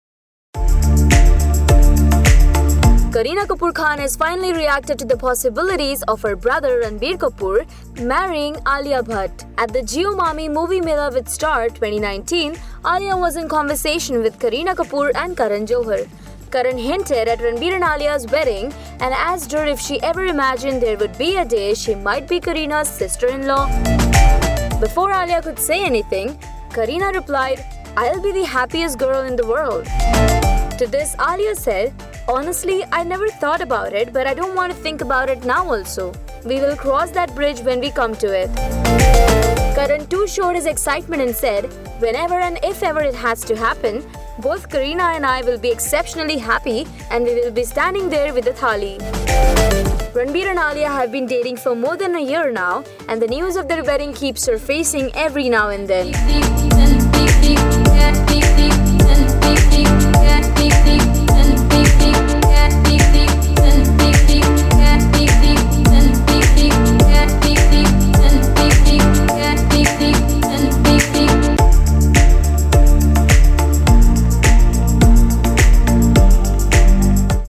Kareena Kapoor REACTS To Ranbir-Alia's Wedding | LehrenTV
Kareena Kapoor Khan has finally reacted to the alliance between lovebirds Alia Bhatt and Ranbir Kapoor. The conversation hints about their wedding.